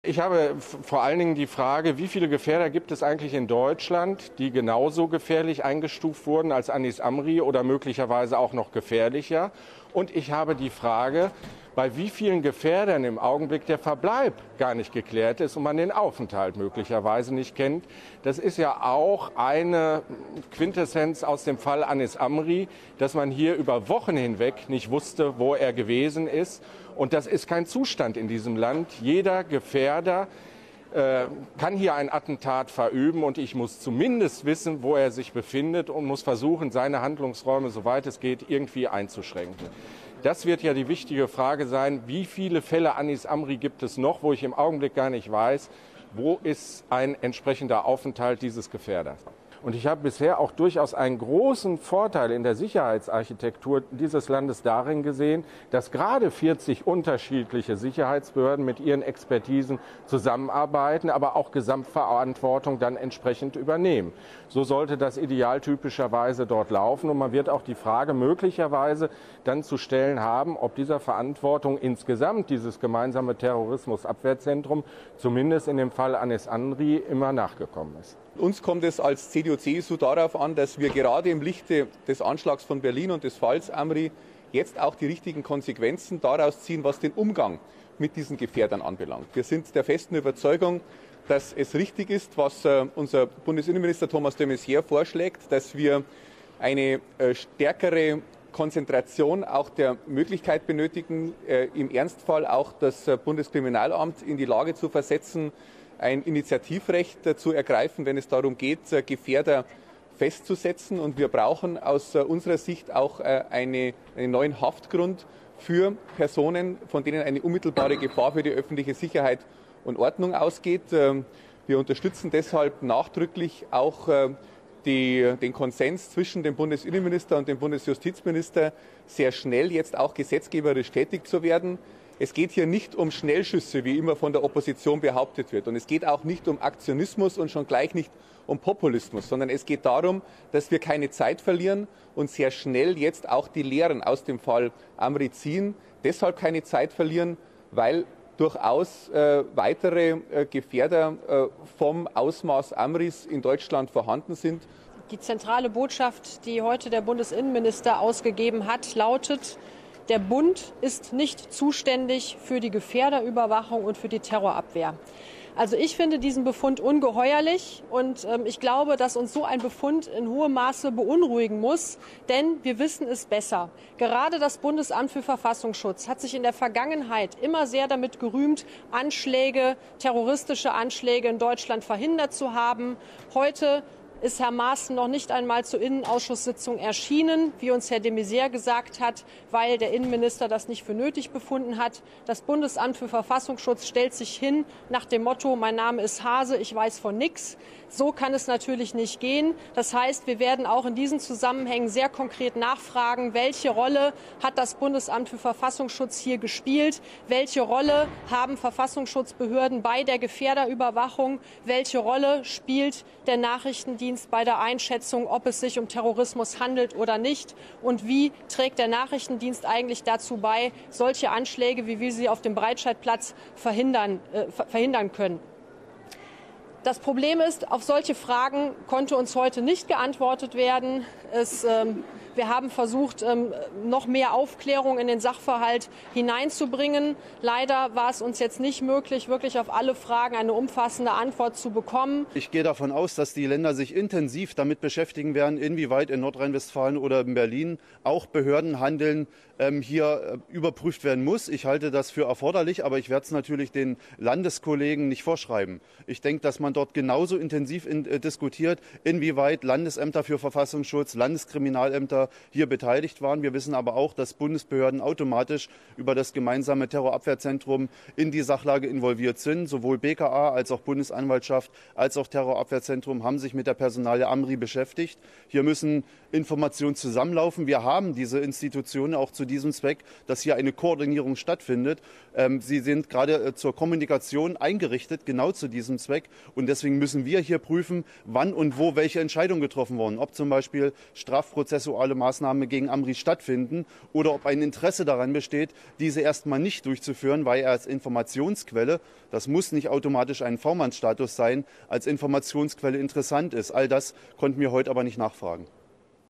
Innenausschuss tagt zu Fall "Amri": Statements nach der Sitzung am 18.01.2017
Nach der Sondersitzung des Innenausschusses äußern sich zum Fall "Anis Amri" Burkhard Lischka (Sprecher für Innenpolitik der Bundestagsfraktion, SPD), Stephan Mayer (Sprecher für Innenpolitik der Bundestagsfraktion, CSU),  Irene Mihalic (Innenpolitische Sprecherin der   Bundestagsfraktion, B´90/Grüne) und Frank Tempel (Stellv. Vorsitzender Innenausschuss, Die Linke).